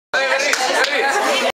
BERRIA. Seaskakoak, PSko egoitzatik irteten
Seaskako guraso eta irakasleek bertan behera utzi dute PSko egoitzaren okupazioa.